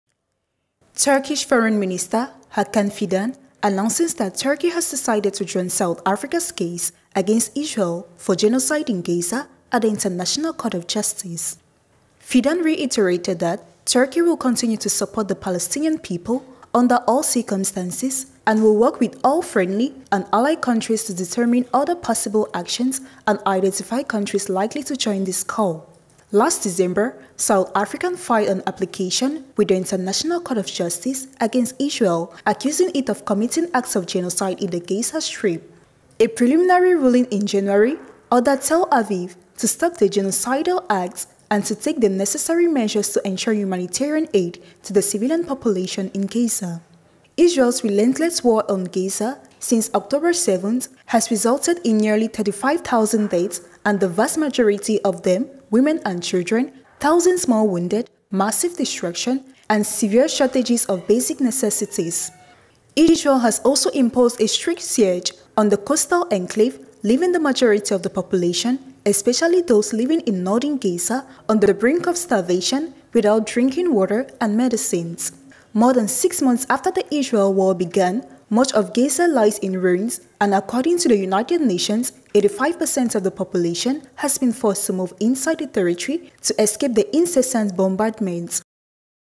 TURKEY PLANS TO JOIN SOUTH AFRICA'S GENOCIDE TRIAL AGAINST ISREAL
Turkey will officially apply to join South Africa's genocide case against Israel at the International Court of Justice (ICJ), the chairman of the Turkish Grand National Assembly's justice committee said on Wednesday. After the decision to join (the case) is made by our esteemed president, we will submit our official application after legal work. As we have done until today, we will continue our efforts with determination to ensure the condemnation of Israel, which has been condemned by the conscience of humanity and also in the eyes of the law.